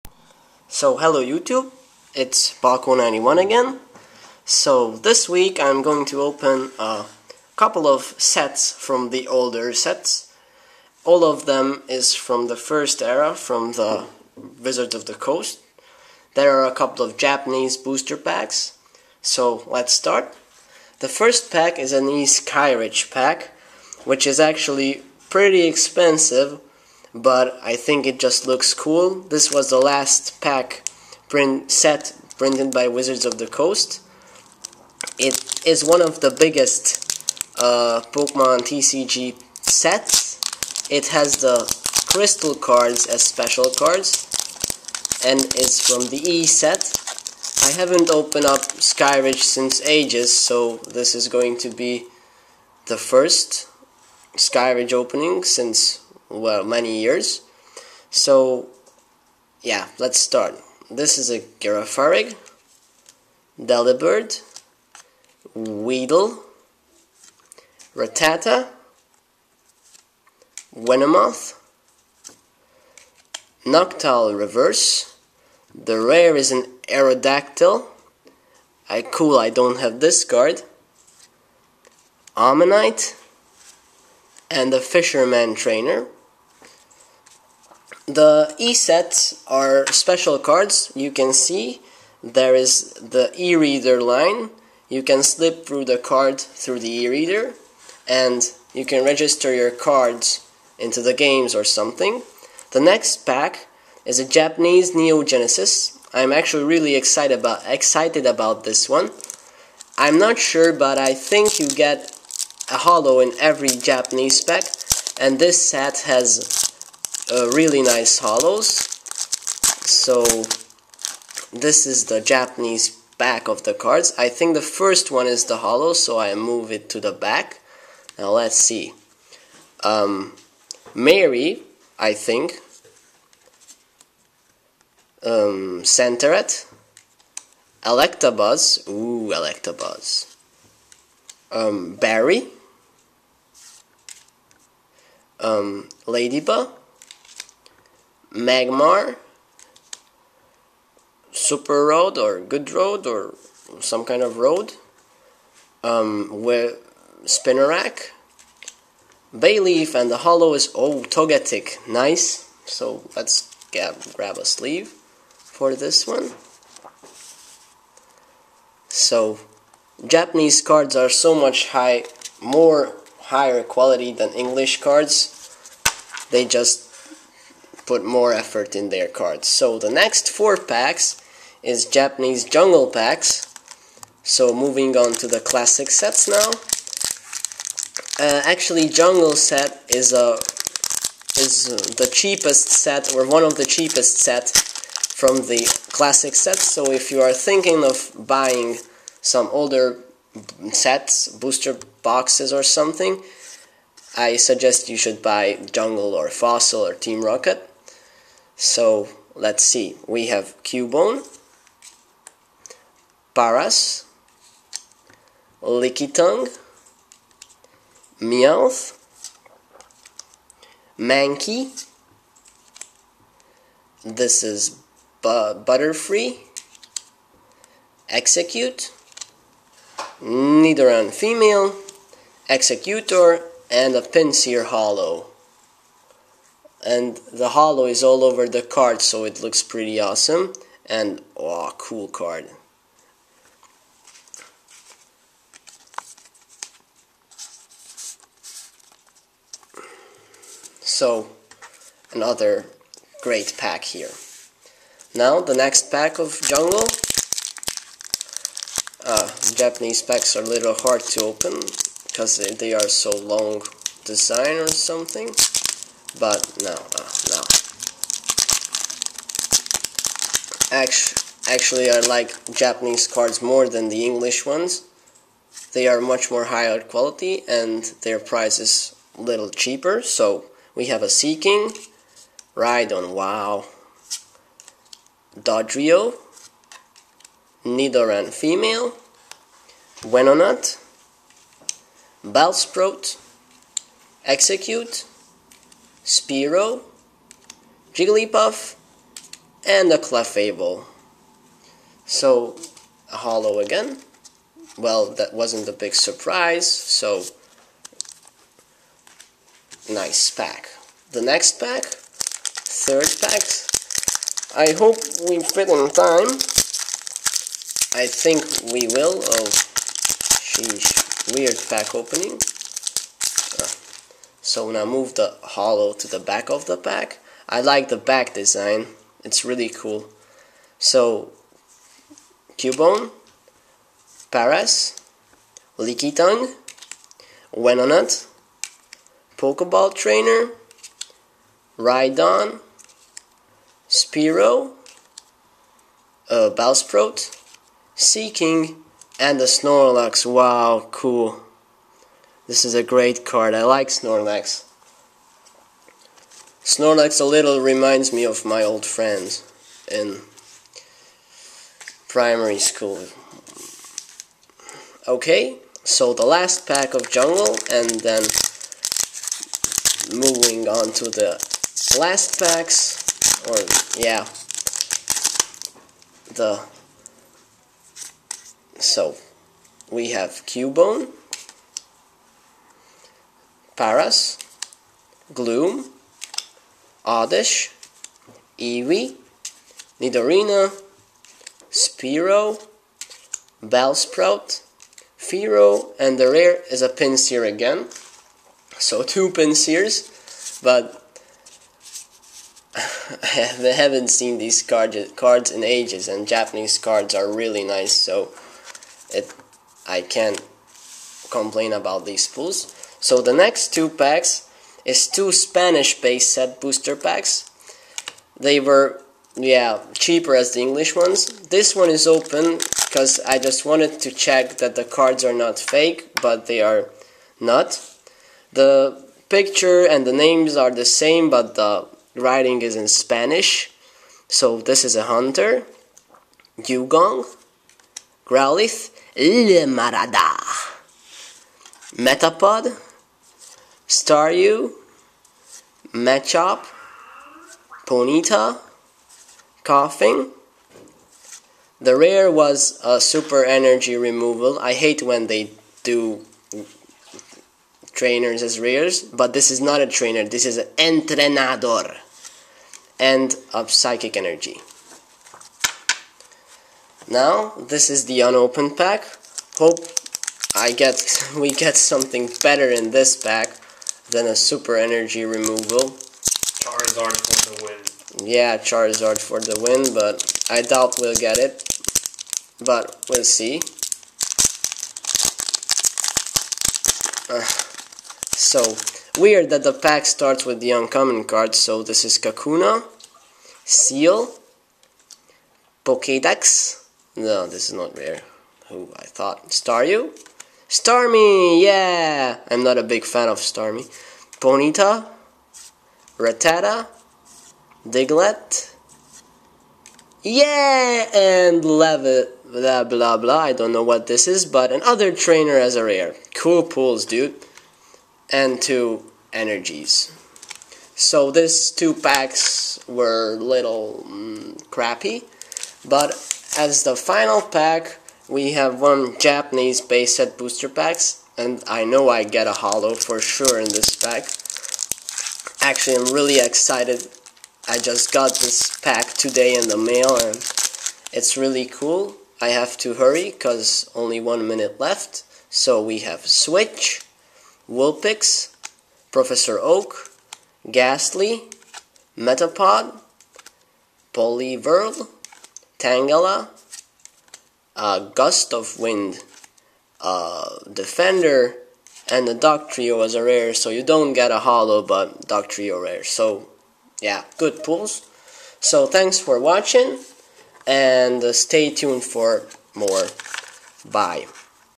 Opening Oldschool Pokemon Booster Packs
Enjoy! Next week I will be using a new camera setup, so hopefully the quality will be noticeably better.